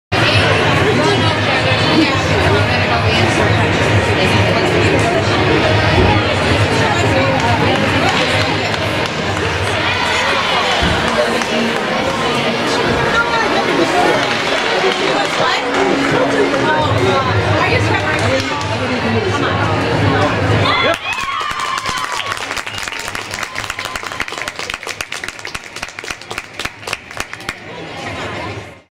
Amy Wozniak, L9 Bars, 9.775, 1st Place, IL State Championships
Illinois State Meet
March 17, 2018
West Chicago, IL